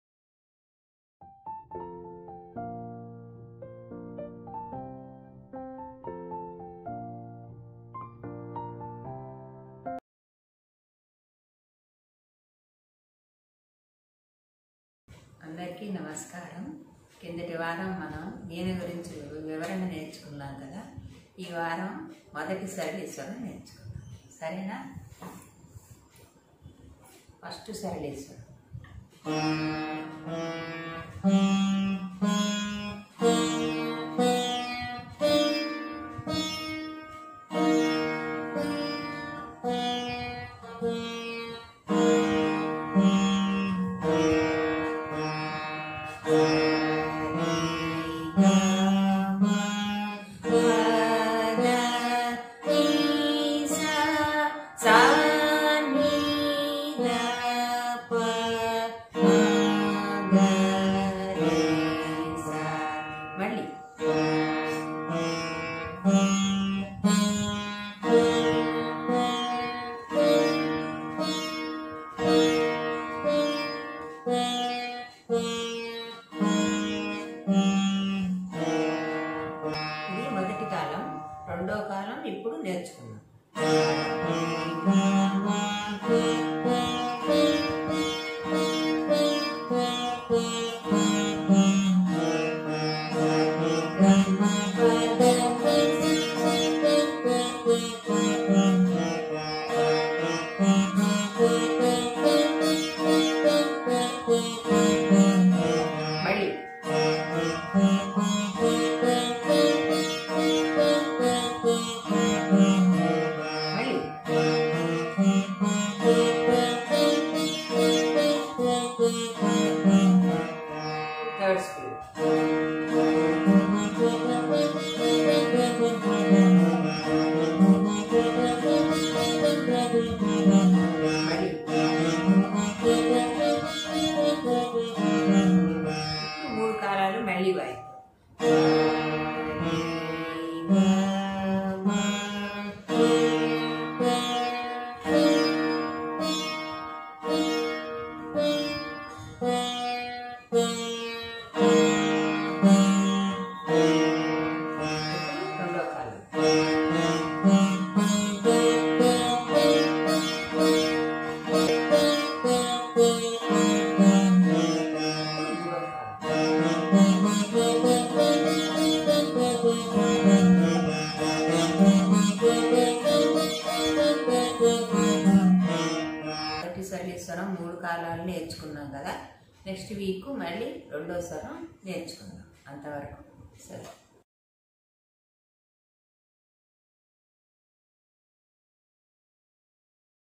SARALI SWARALU ON VEENA - SWARA 1
Hello viewers,

SARALI SWARAS -1
Raga: Mayamalavagowla (15th Melakartha Ragam)
Arohana: S R1 G3 M1 P D1 N3 S
Avarohana: S N3 D1 P M1 G3 R1 S
Talam: Adi